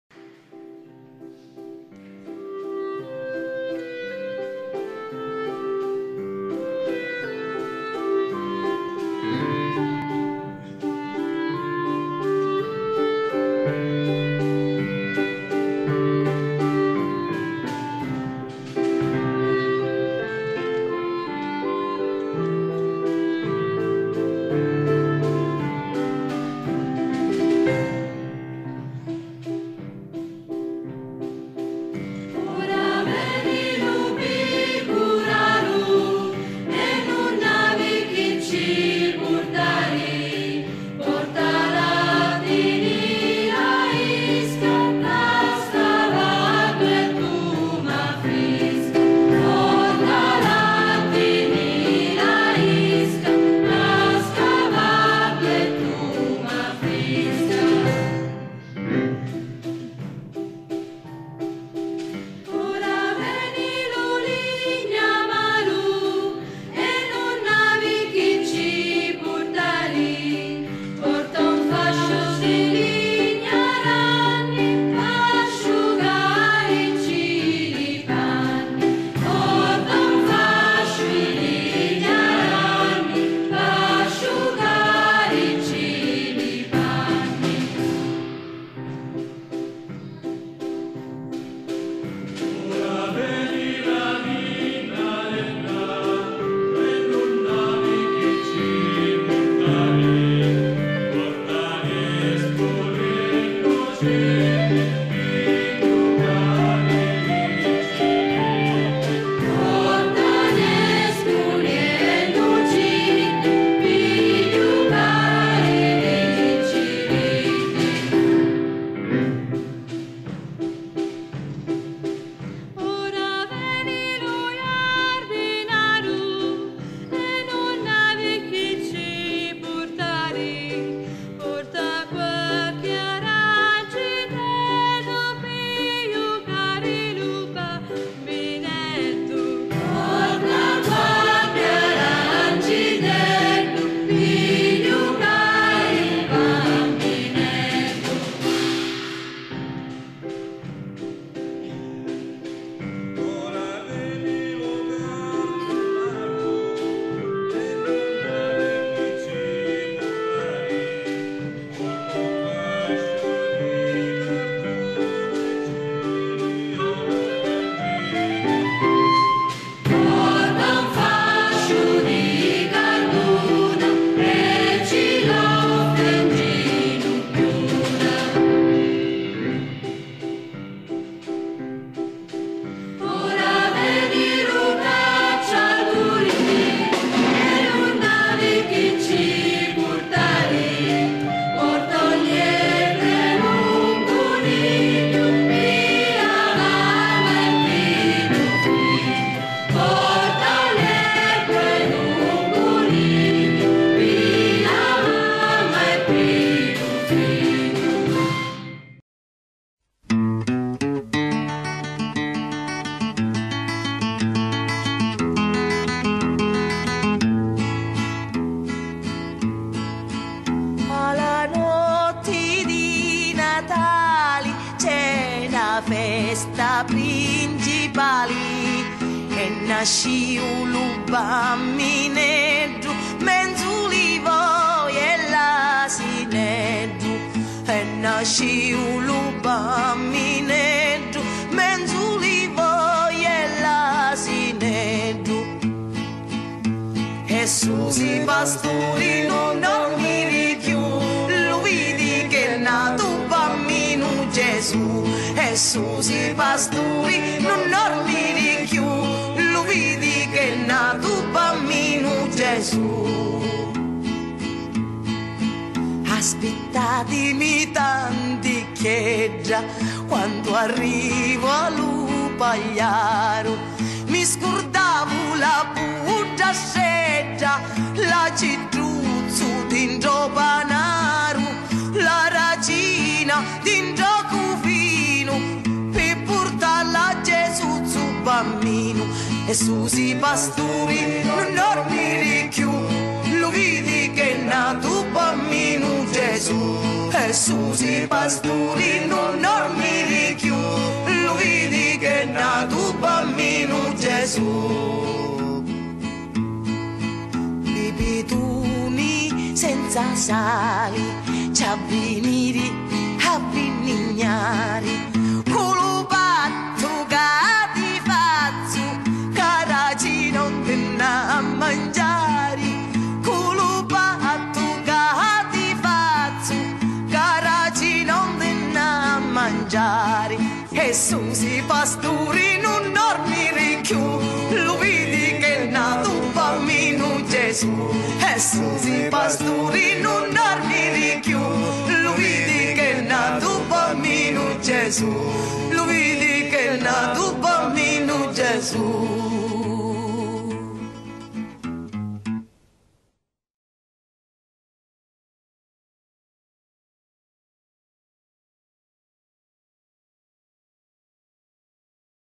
presepe vivente San Paolo 2015
presepe vivente organizzato dalla proloco di palazzolo acreide nel quartiere di san paolo